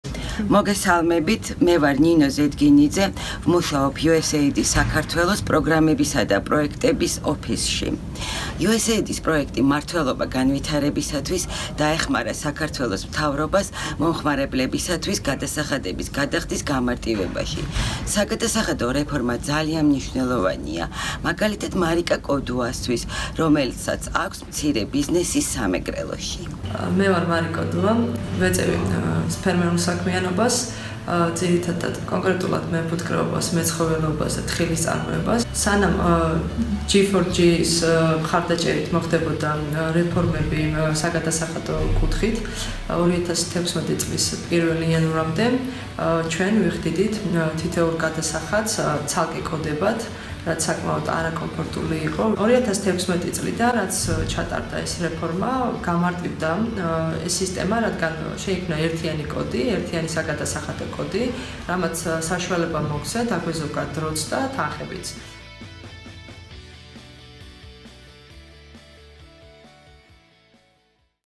USAID's Tax Simplification Assistance
USAID/Georgia helped the Georgian government to simplify its tax filing system. For example, before the tax reforms there were 125 treasury codes to choose from, leading to confusion and mistakes. Now, there is just 1. This kind of reform is crucial for small business people like Mari Kodua, a farmer in Samegrelo.